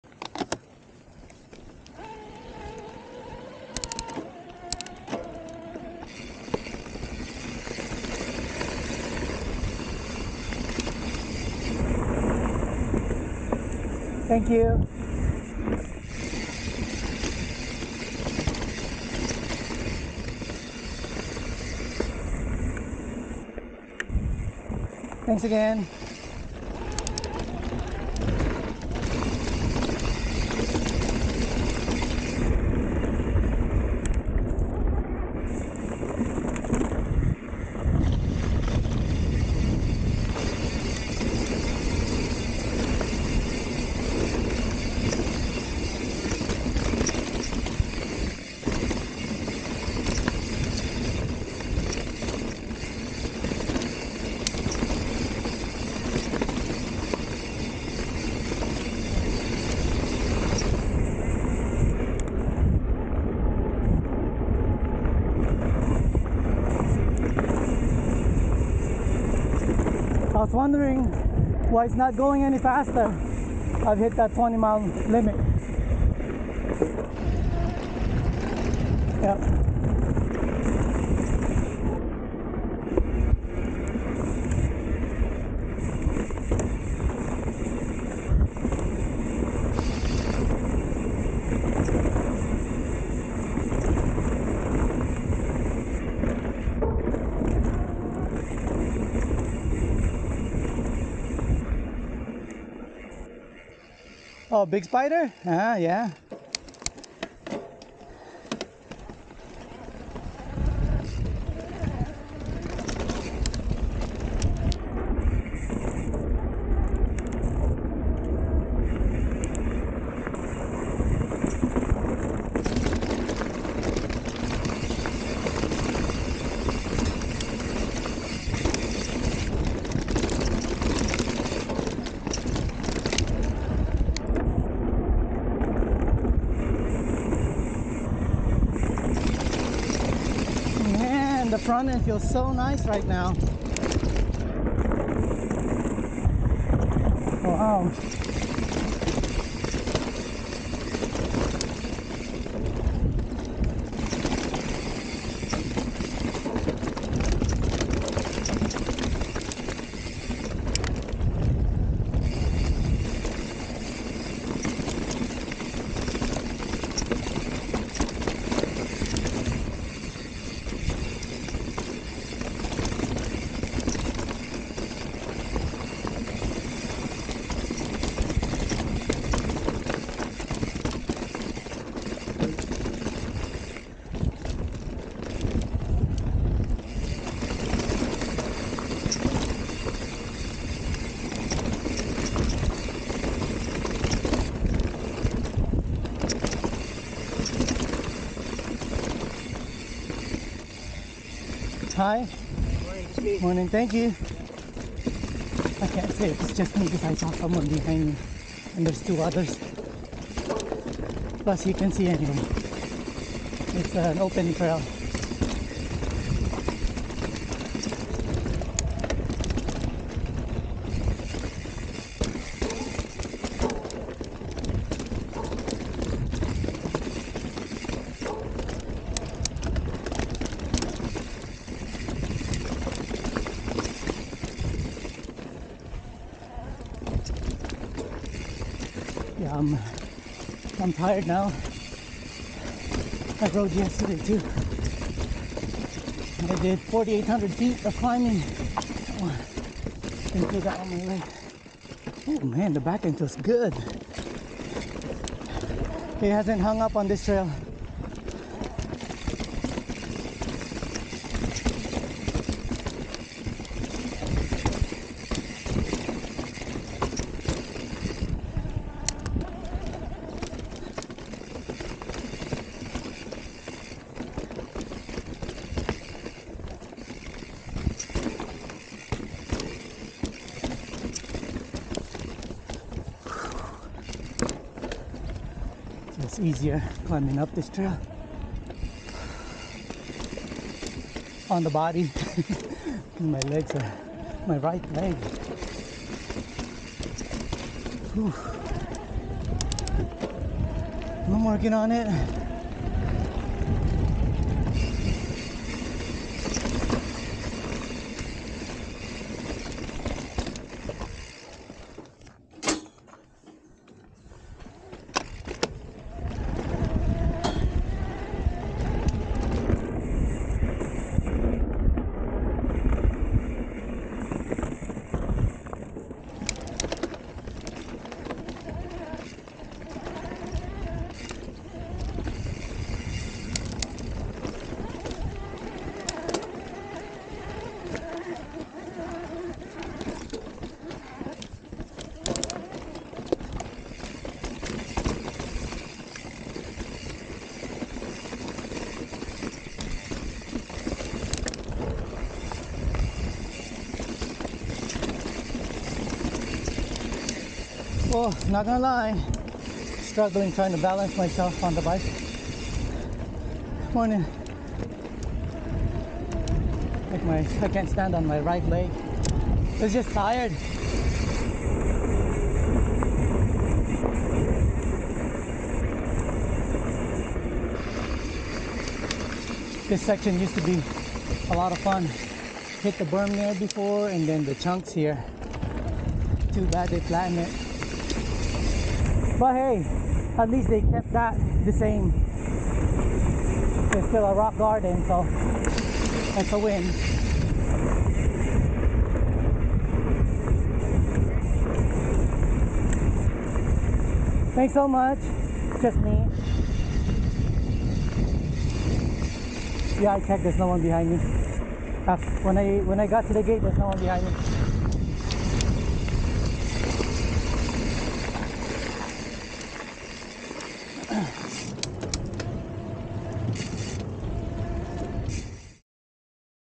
Trying out the new suspension settings
Took a while but finally here!

I swapped out the air shaft from Debonair C1 160 to Debonair B1 170. B1 so that I get the small bump compliance that the C1 didn’t have. 170mm because the actual travel on the fork is 160mm on static weight.

Fox Floax X R tune - removed 10 psi from last ride here and rode this at 8 clicks Rebound.

I bought some parts to make the rear end even better. Its going to take about a month before I get it and I can't wait to try it on this trail! Hopefully it gets here before the rain comes or else I can't ride this trail. Usually this trail is closed when wet.